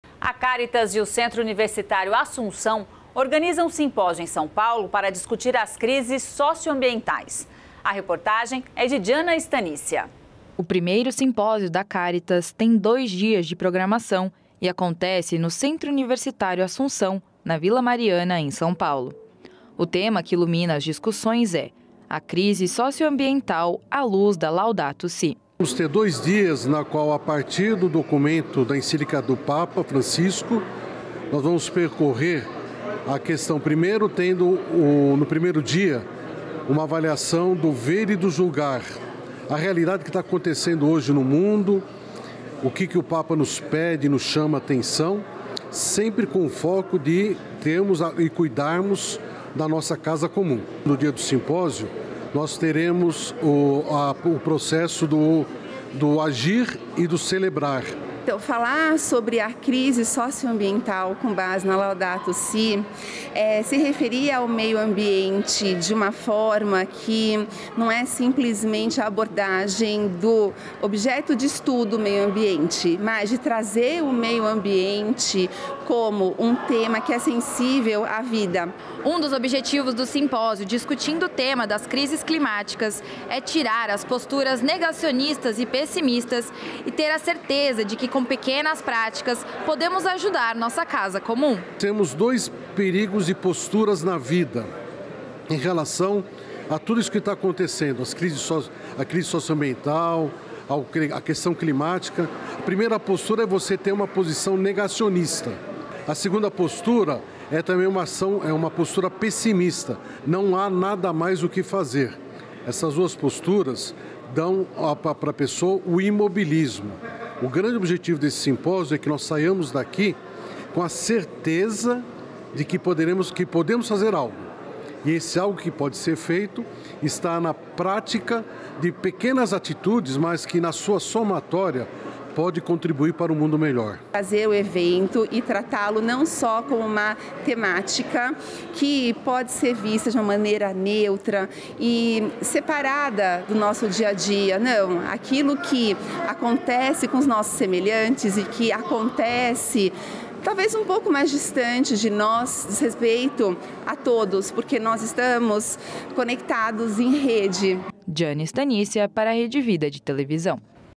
1º Simpósio da Cáritas discute a crise socioambiental – JCTV – 28/05/24
Cáritas e Centro Universitário Assunção organizam Simpósio em São Paulo para discutir as crises socioambientais. A reportagem é de Gianna Staniscia.